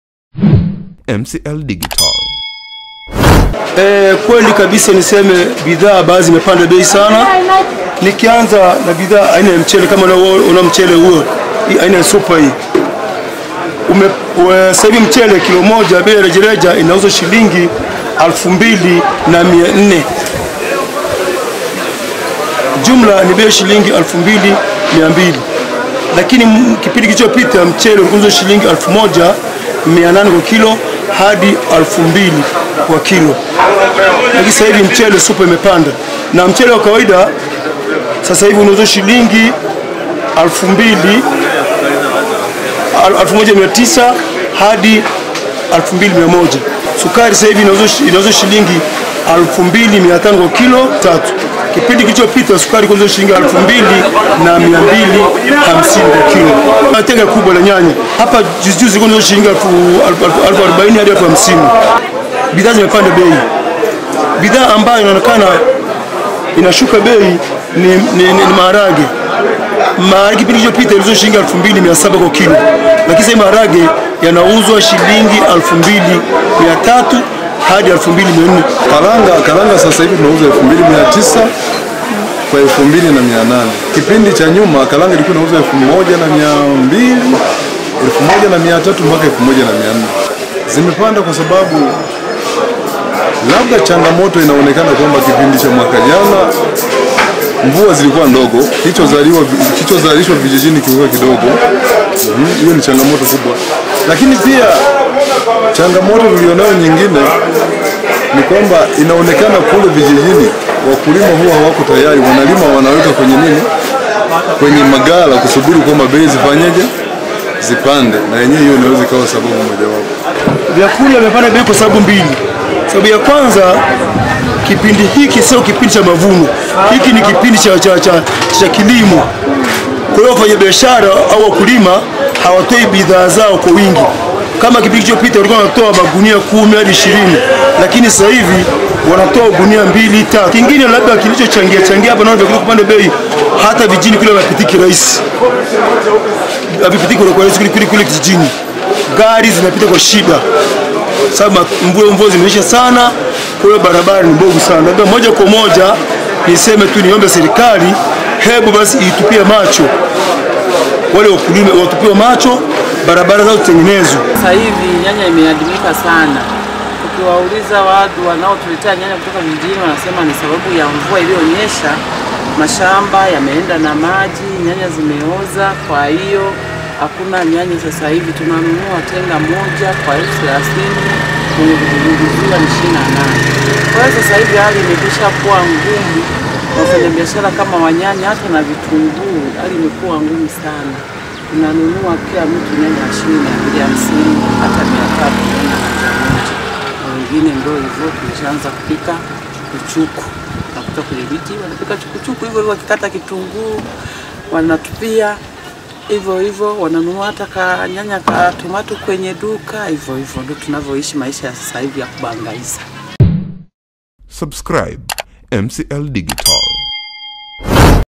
BEI ZA NAFAKA/VIUNGO ZAPANDA, WANANCHI WAGEUKIA CHUKUCHUKU
Baada ya mazao ya nafaka kupanda bei mkaoni Mwanza, wafanyabiashara wa nafaka wametaja sababu za kupanda kwa bei hizo huku sababu kubwa ikionekana ni mvua zinazoendelea kunyesha.
Kwa mujibu wa Mwenyekiti wa Soko Kuu la Mwanza Ahmad Nchola amesema, bidhaa zilizopanda ni Mchele  kutoka Sh1,800 kwa mchele supa kufikia 2,400 kwa kilo moja huku mchele wa kawaida ukipanda kutoka Sh15,00 kufikia Sh1,850.